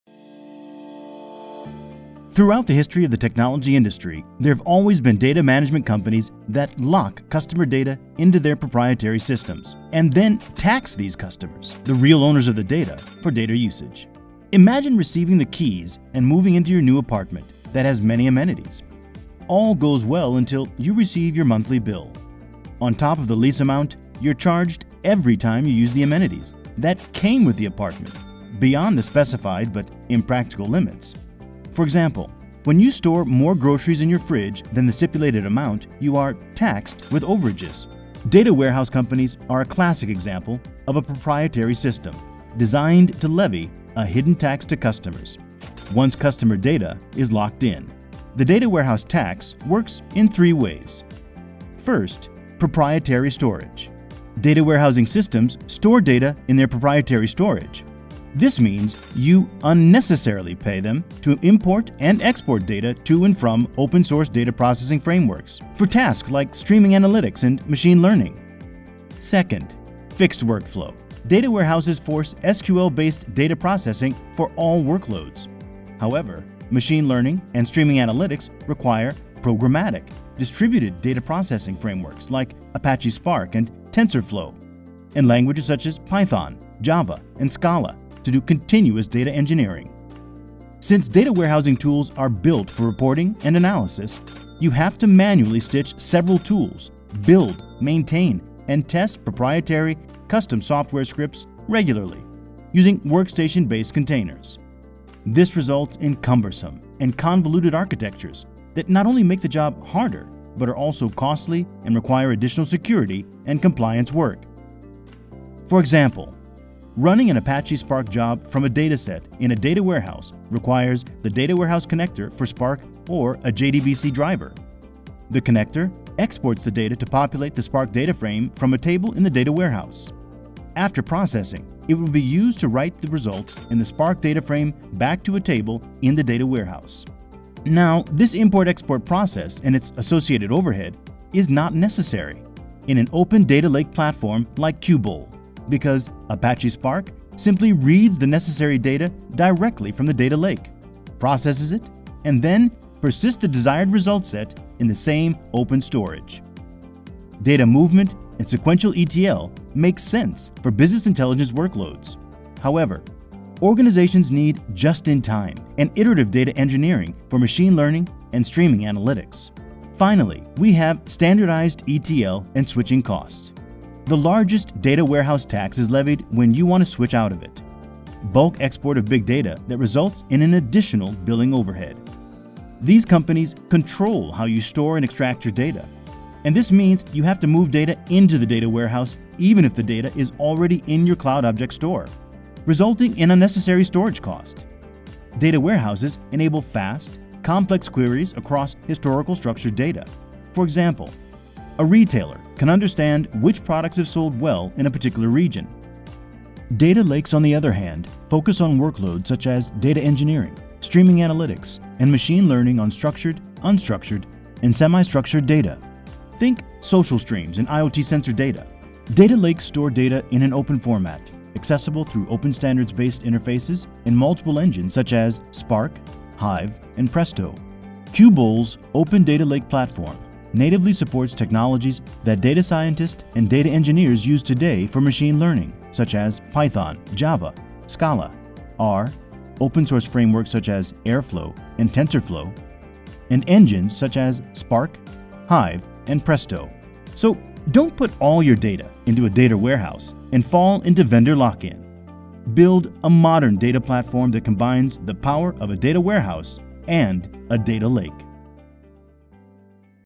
Say no to the Data Warehouse Tax
History of the Software industry is littered with examples of data management companies that lock customer data into their proprietary systems, and tax those customers for data usage. Data warehouse companies are a classic example. Qubole’s Data Lake platform, on the other hand, stores data in an open format making it accessible through open standard-based interfaces, preventing vendor lock-in and natively supports technologies that data scientists use today for ML and open-source framework.